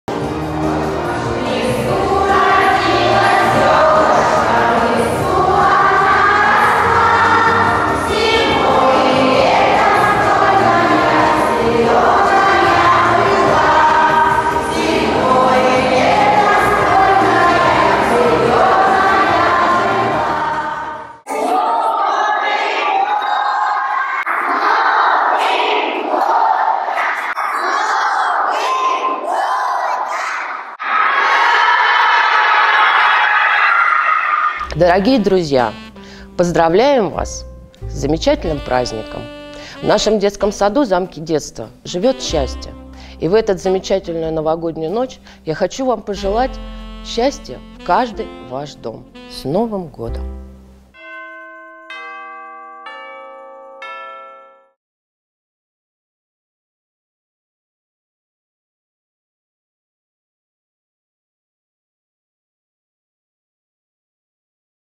Замок детства в Совхозе Грудинина. Новогоднее поздравление 2020
В Совхозе имени Ленина есть замечательные замки - это детские сады, которые построил Павел Грудинин на средства народного предприятия ЗАО "Совхоз имени Ленина". Директор "Замка детства" и детишки поздравили всех с наступающим Новым годом.
Помогите каналу: Карта СБ - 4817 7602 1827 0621